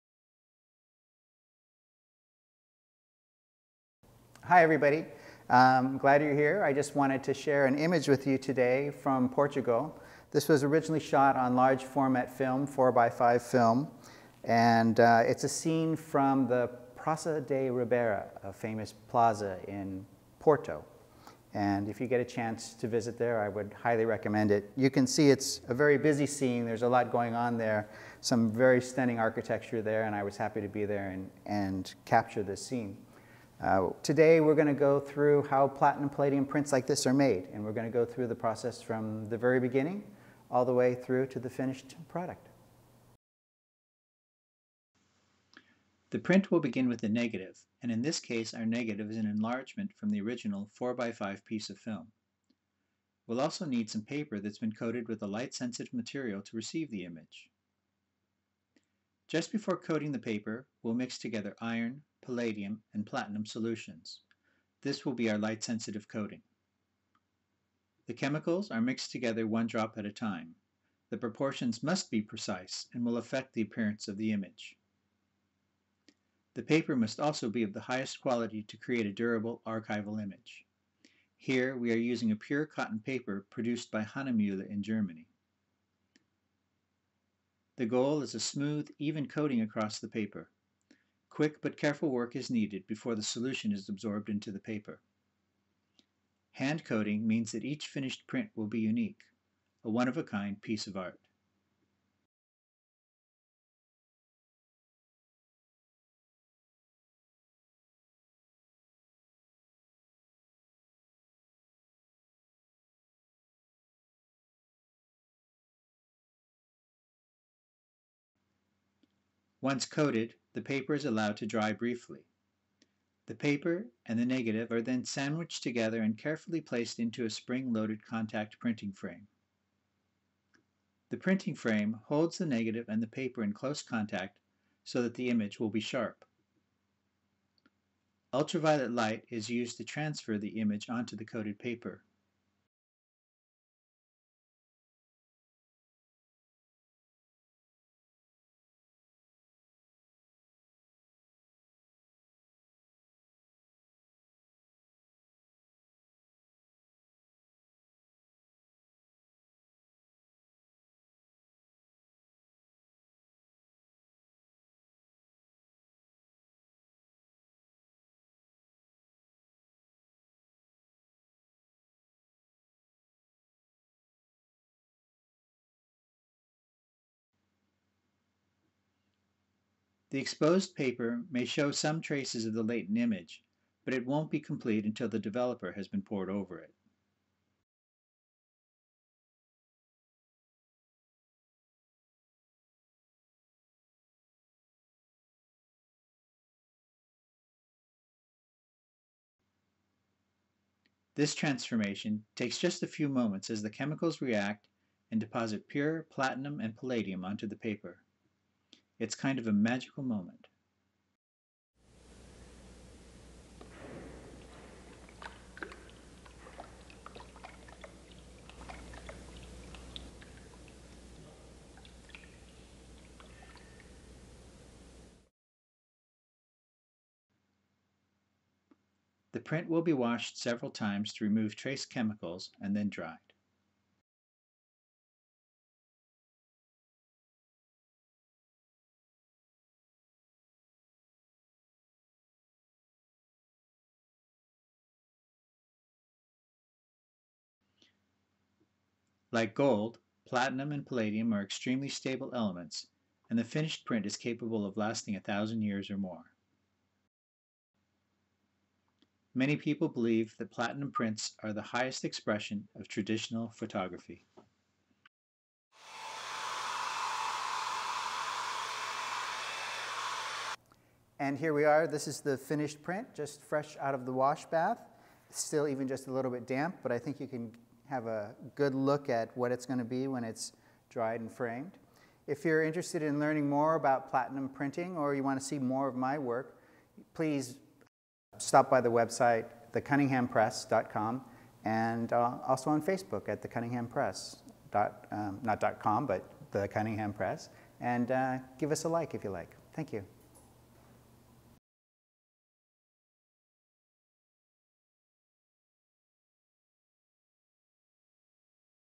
Making a Platinum Palladium Print with Paul Cunningham
This HD video shows the steps involved in creating a handmade platinum print. (Closed captions in English.)

Videography by Chas Stiltner

Materials used include:
· Platinum Rag fine art cotton paper by Hahnemühle
· Platinum palladium chemistry from Bostick & Sullivan
· Jack Richeson 9000 series brush

Filmed on location at Beast Daylight Studio (Buck East) in Portland, Oregon. Edited in Adobe Premiere Pro CC.

Special thanks to master platinum printer Ray Bidegain, and the entire Portland photographic community for their continuing encouragement and support.